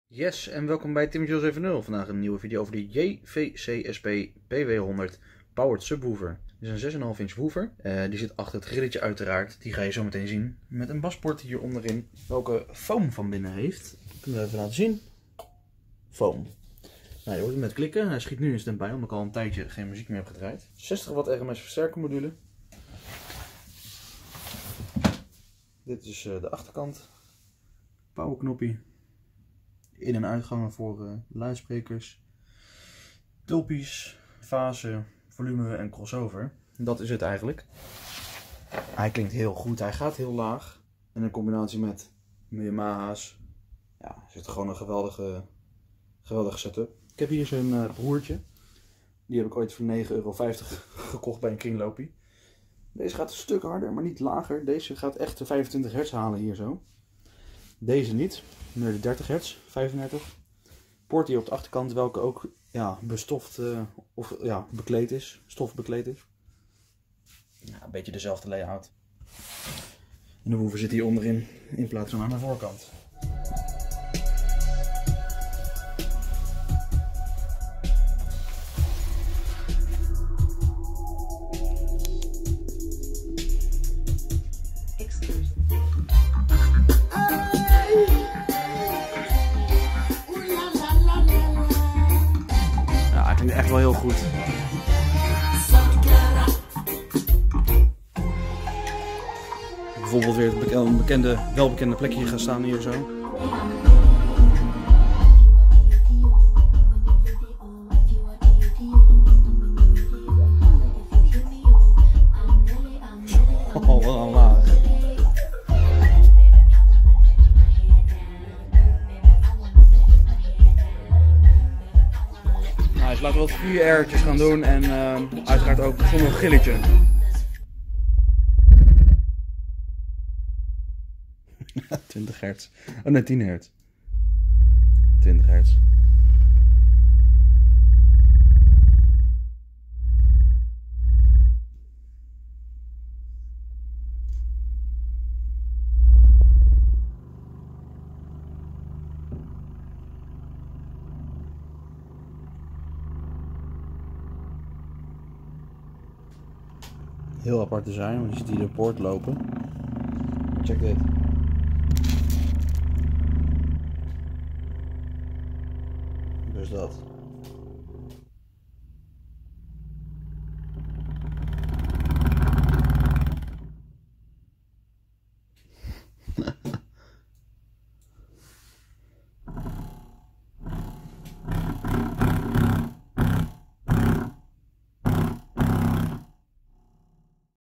JVC SP-PW100 Powered subwoofer!
Gekocht voor 10 euro, omdat deze bij de buren stond toen ik een jaartje of 8 was. Ik was altijd onder de indruk hoe deze klonk.
Nu, 15 jaar later zelf een gescoord en snap ik weer waarom deze zo goed was.

Yooo Tof dat je kijkt naar deze video!
Ik ben Timmetjuh070, gek op audio apparatuur en heel toevallig vind ik het ook leuk om video's te maken!